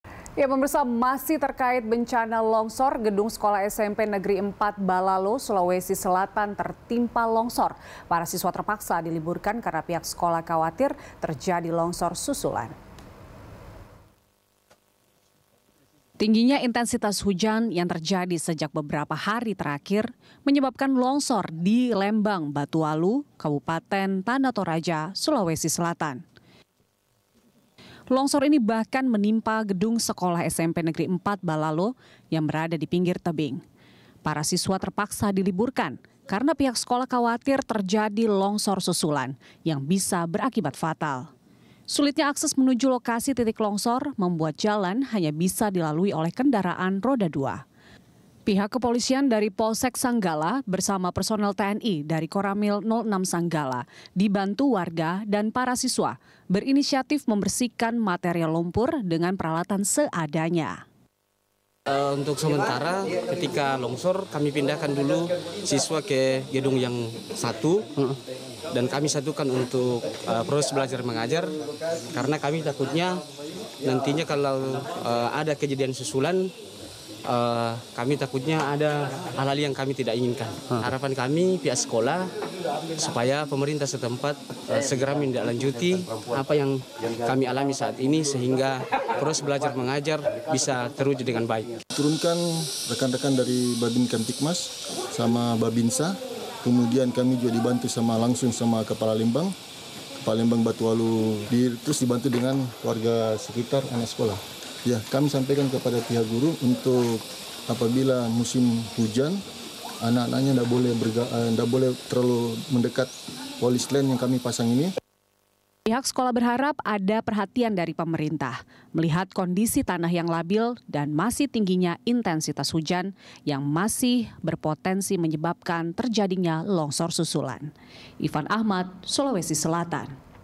Gedung SMPN 4 Balalo Tertimpa Longsor, Siswa Diliburkan
Gedung SMPN 4 Balalo, Sulawesi Selatan, tertimpa longsor. Para siswa diliburkan karena pihak sekolah khawatir terjadi longsor susulan.